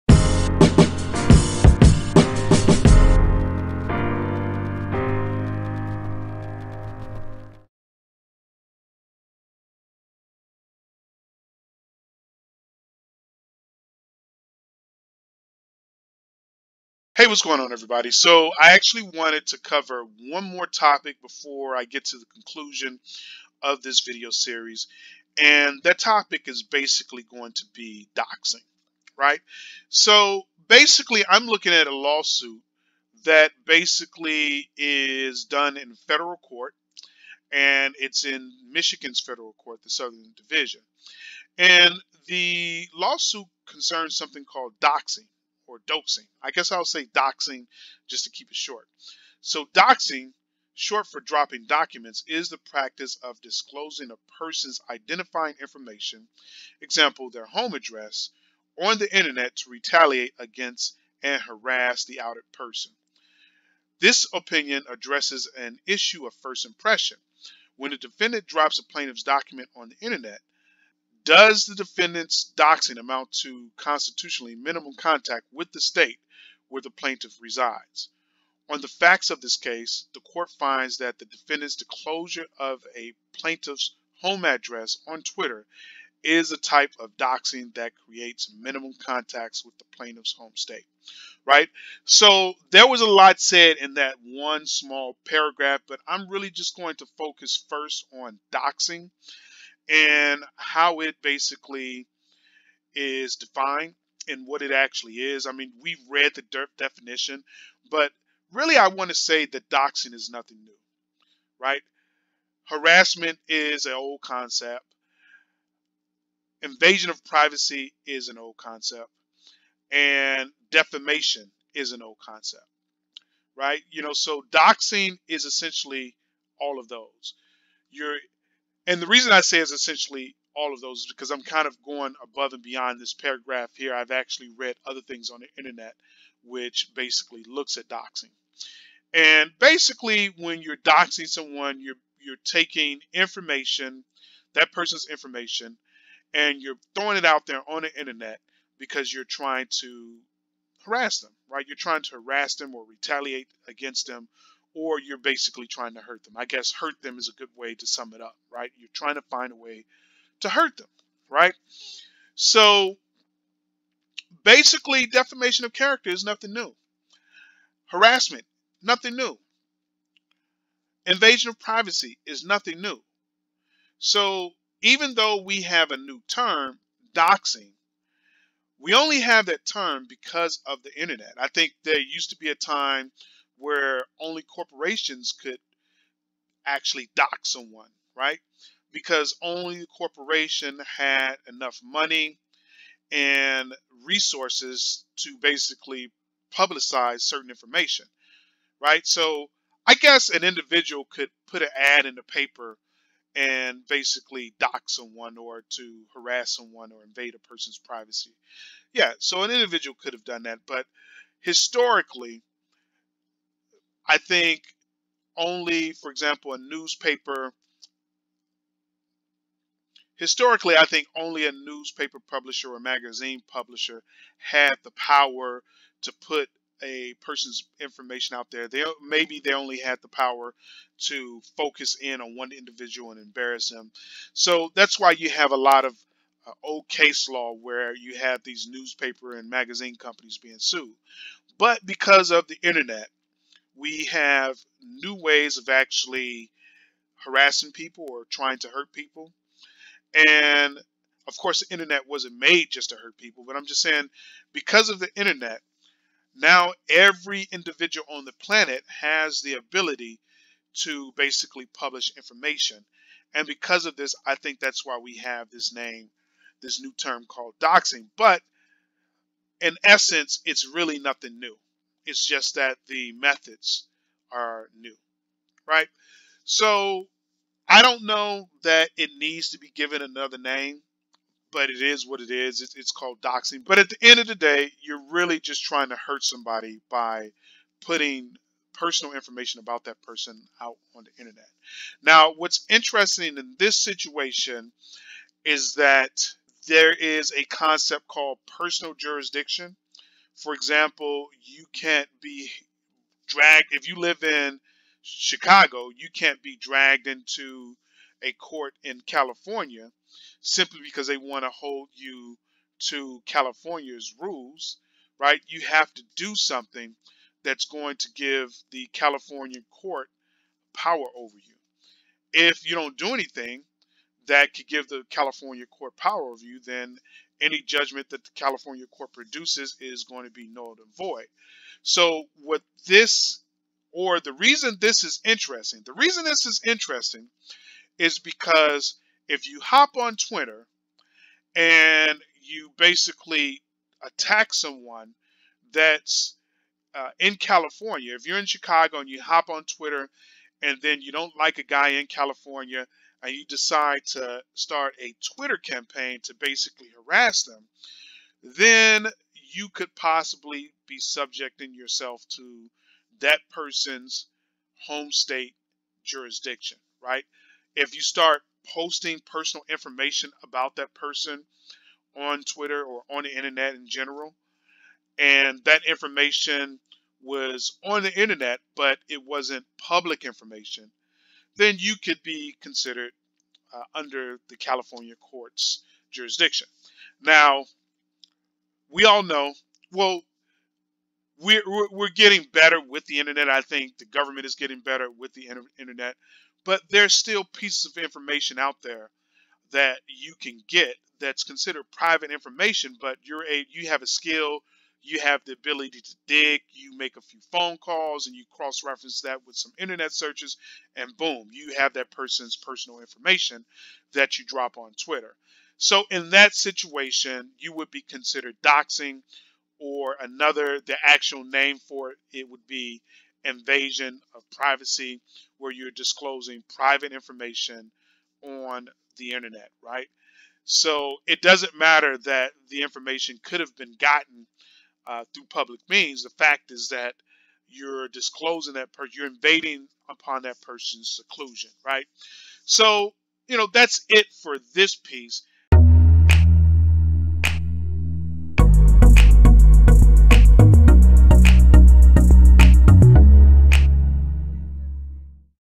Doxing
This video covers a new tort called Doxing.